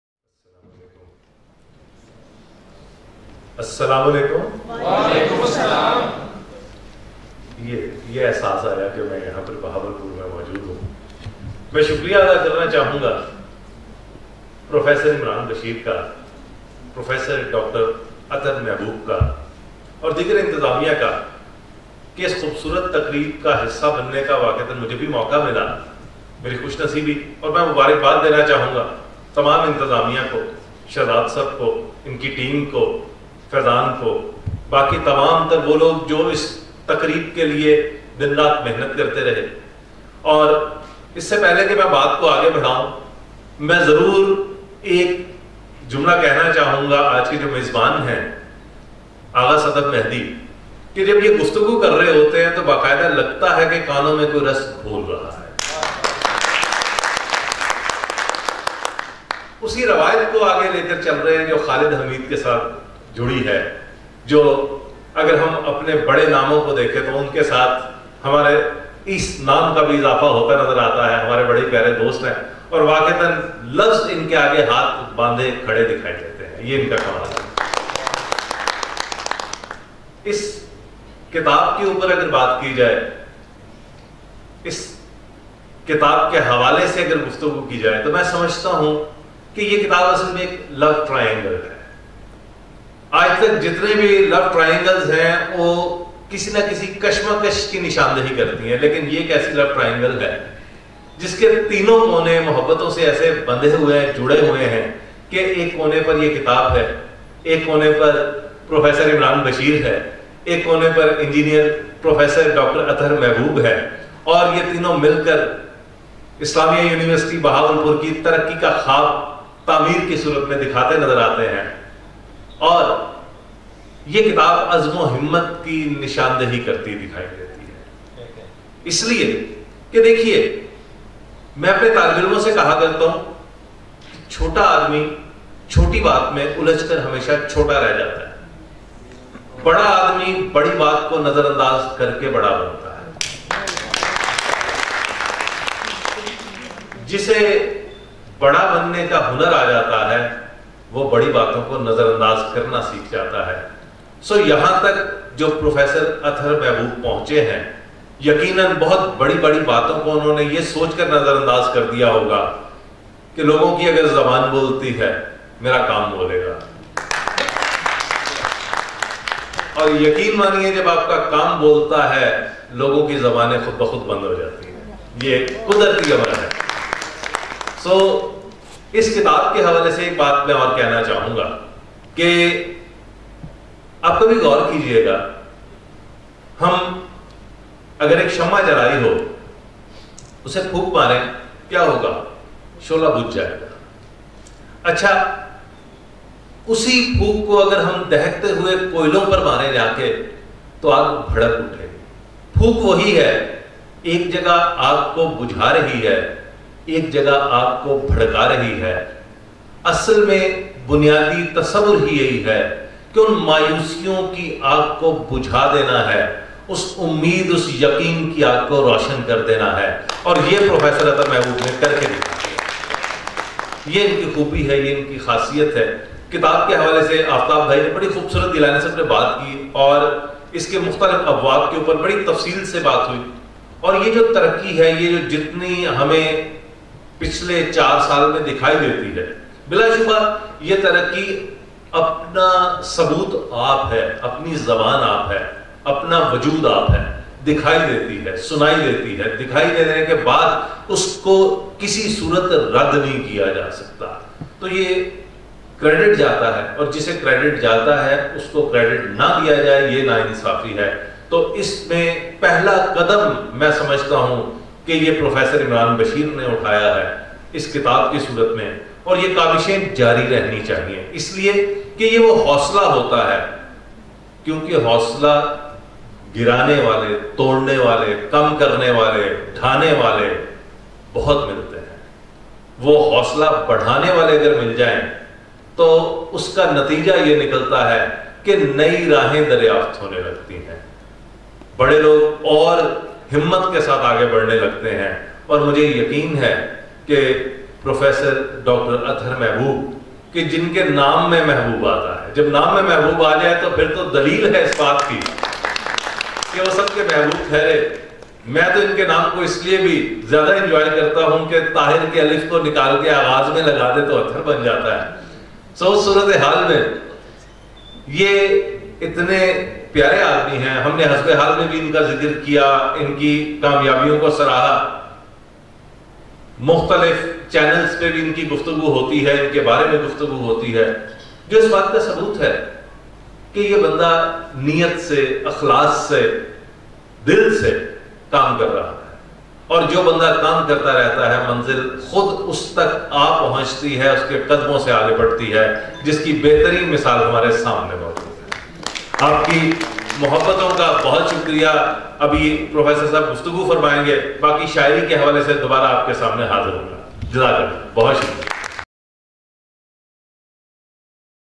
Address of Dr. Tahir Shaheer | Renowned Poet | Book Launch Ceremony of "ستاروں پہ جو ڈالتے ہیں کمند"
The Islamia University of Bahawalpur organized a Book Launch Ceremony of the book “ستاروں پہ جو ڈالتے ہیں کمند “of Author Prof. Imran Bashir on Saturday, July 15, 2023 at Ghulam Muhammad Ghotvi Hall, Abbasia Campus, the Islamia University of Bahawalpur.

This is official YouTube channel of the Islamia University of Bahawalpur